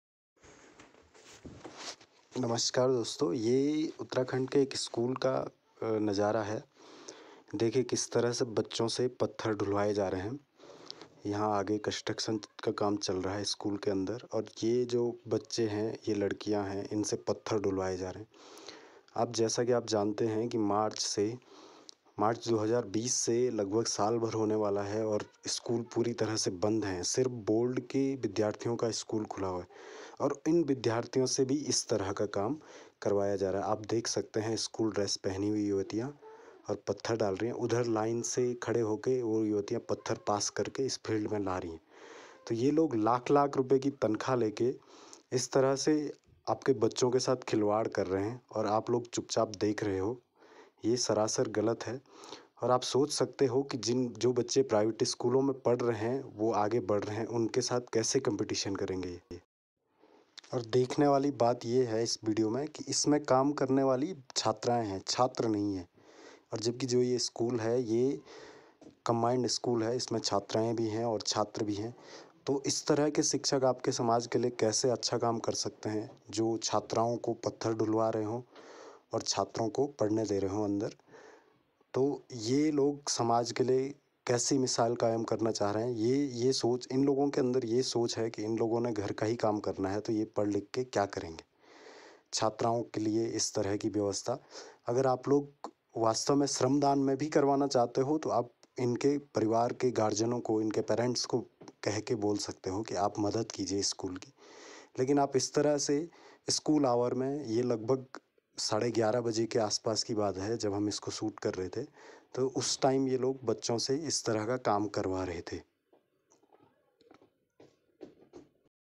aatmanirbharbharat atmanirbharuttarakhand सरकारी स्कूलों में आत्मनिर्भरता के गुर सीखे विद्यार्थी
आने वाले भविष्य की तैयारी के लिए बच्चे हो रहे तैयार